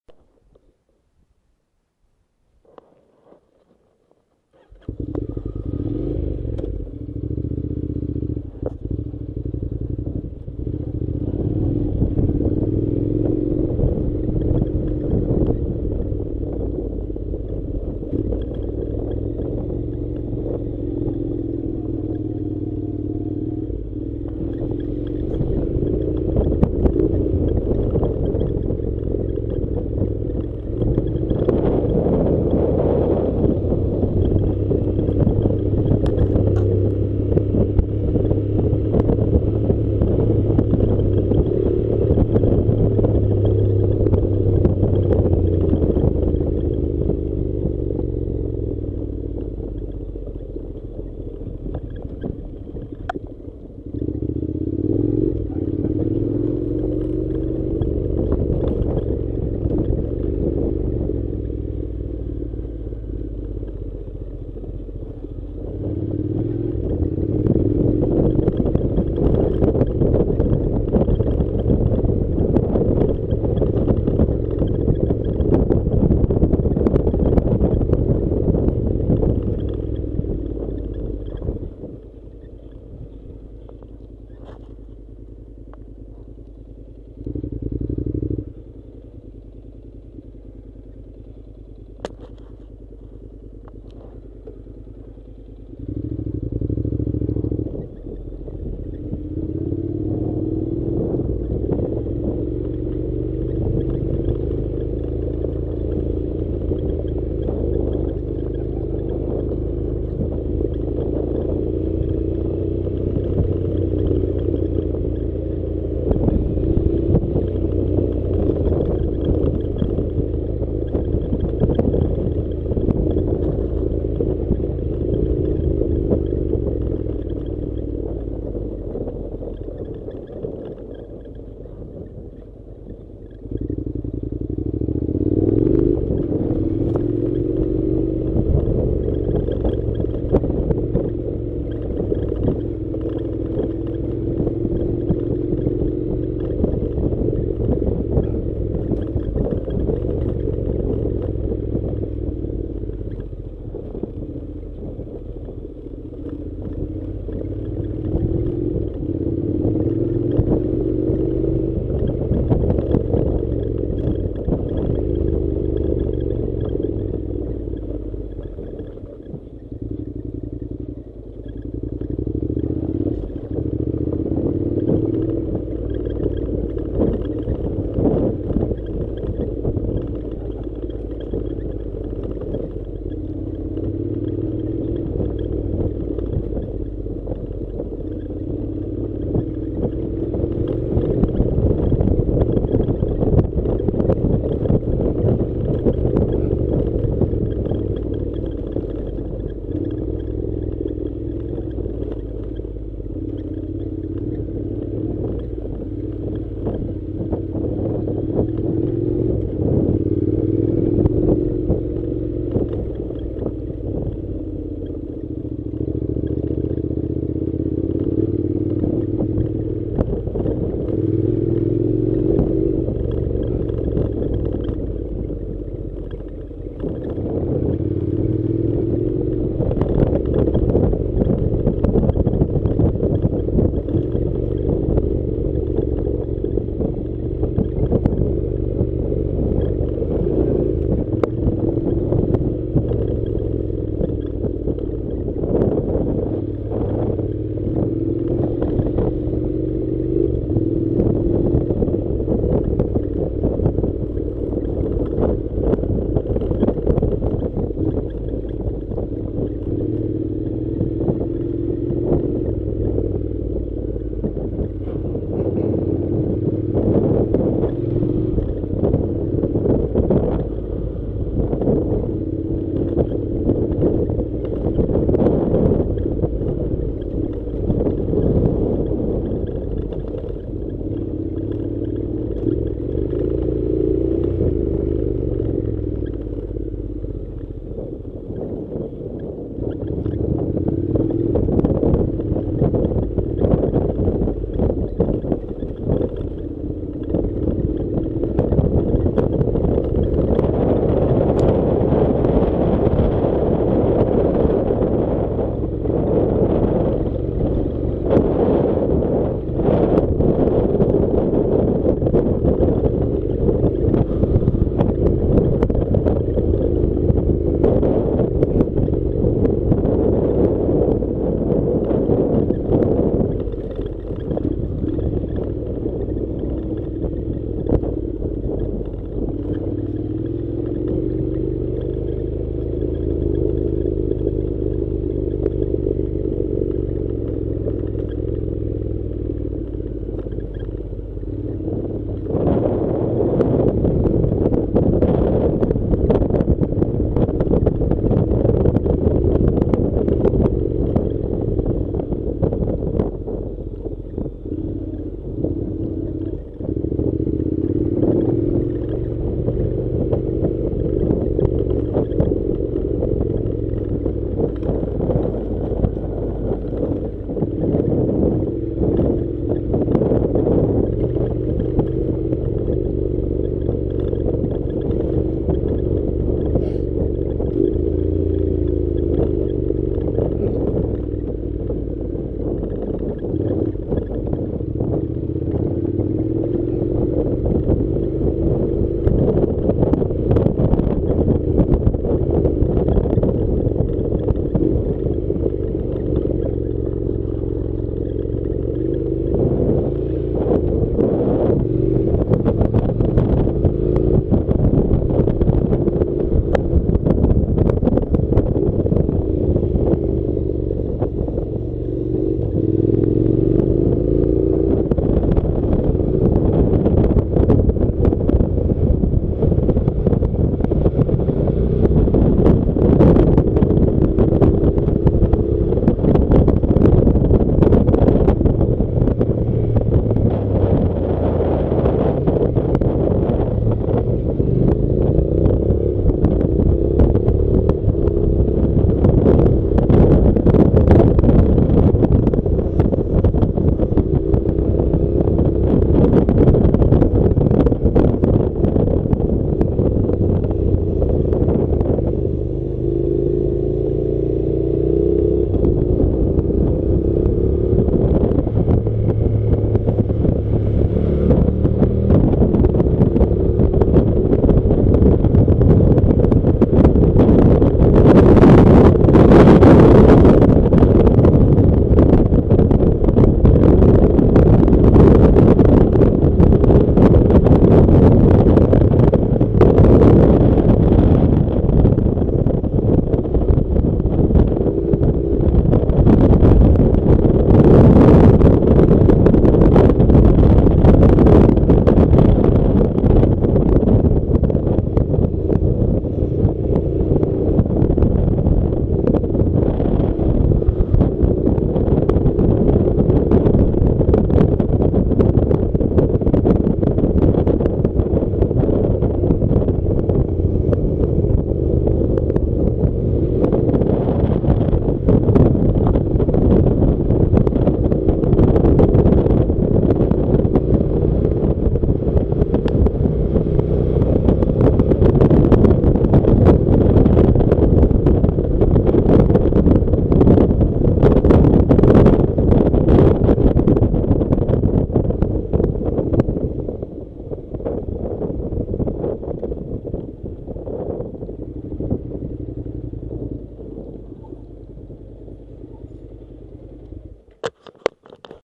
Honda NC700 Αετοί-Αστροχώρι off road
Απο τις στάνες κινήθηκα βόρεια κάνοντας ακόμα μια υπέροχη χωμάτινη διαδρομή η οποία με έβγαλε στο Αστροχώρι.

From the sheepfolds I moved north making another wonderful dirt route which took me to Astrochori.